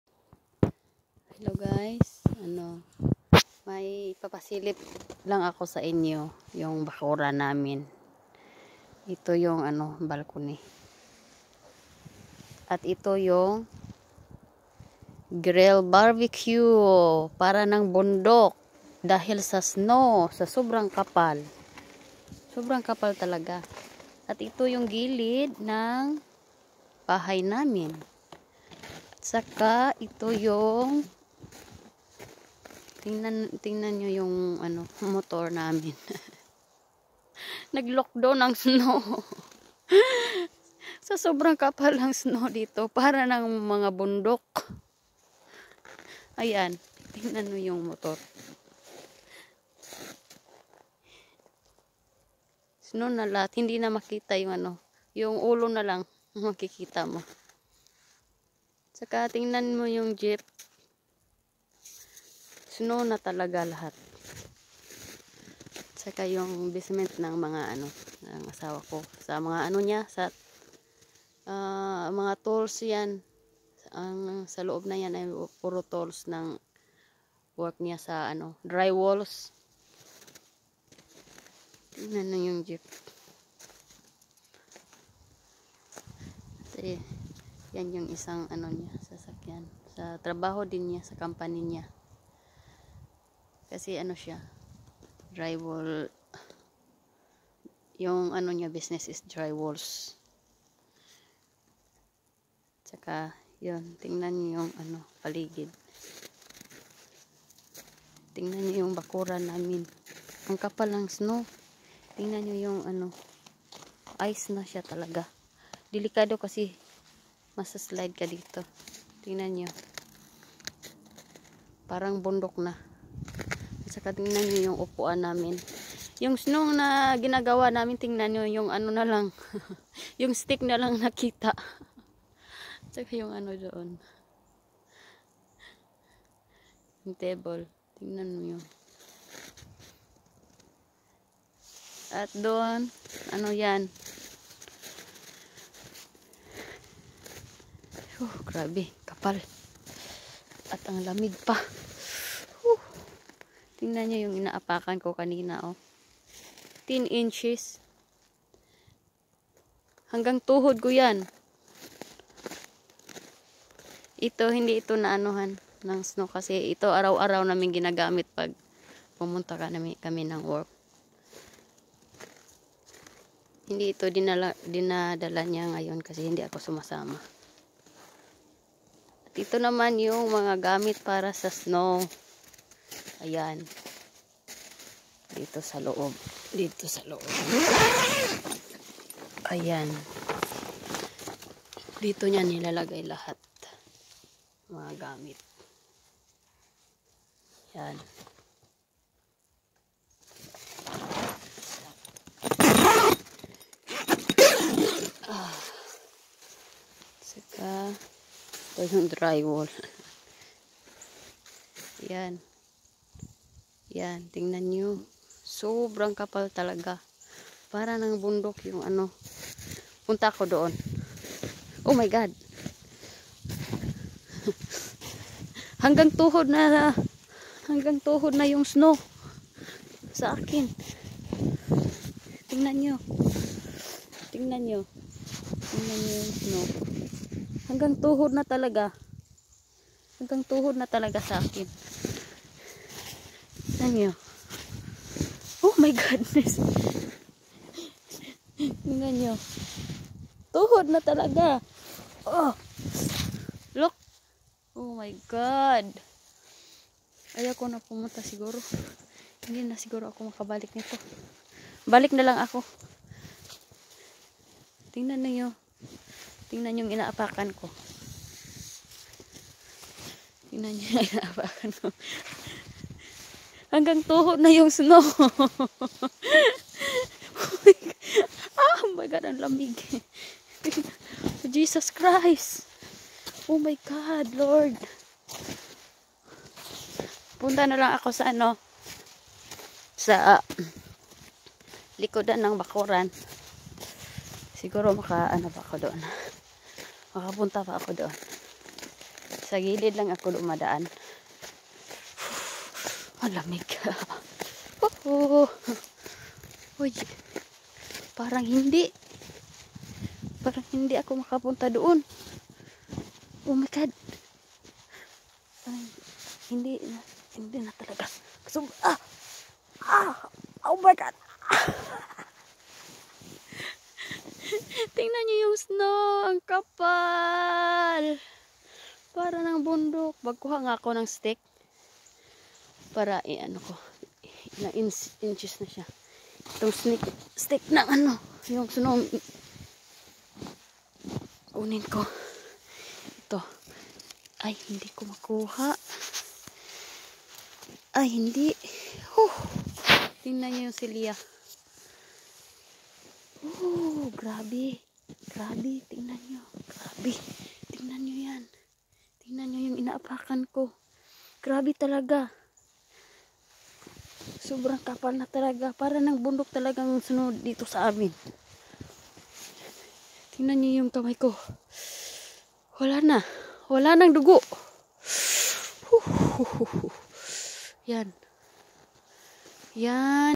Guys ipapaslip kulang kong gaano kakapal ng snow❄ sa bakuran namin.
Sa subrang kapal ng snow para ng bundok⛰